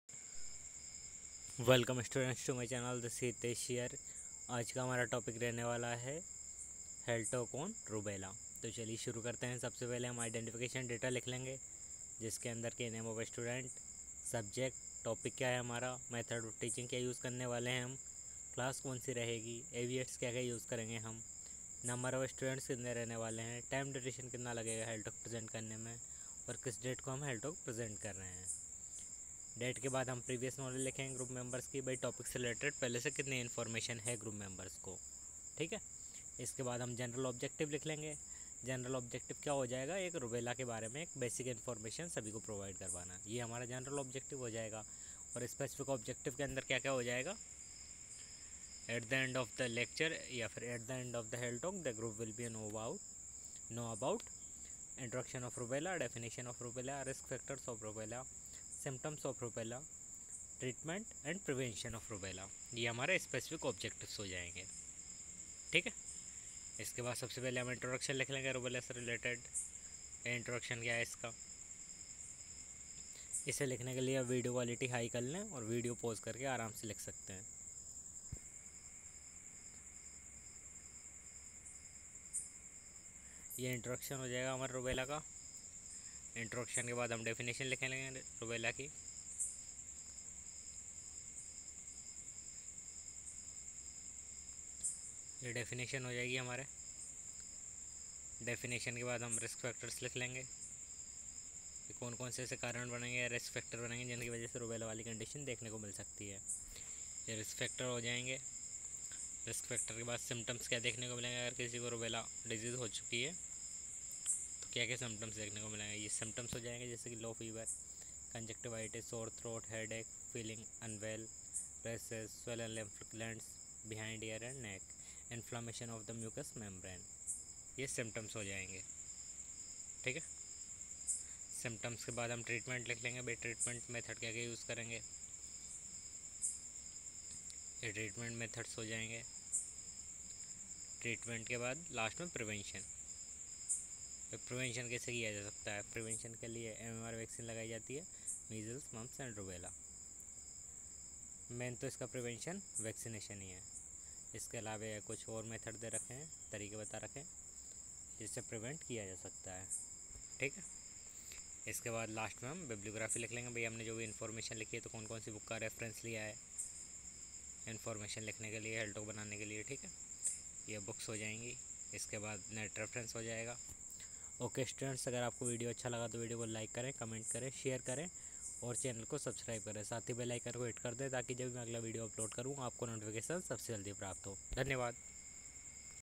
Health talk on Rubella // Pediatrics/msn/chn // for nursing students // @Dhyeyanursingacademy 🩺
This video provide knowledge about how to make health talk on Rubella in a simple and easy way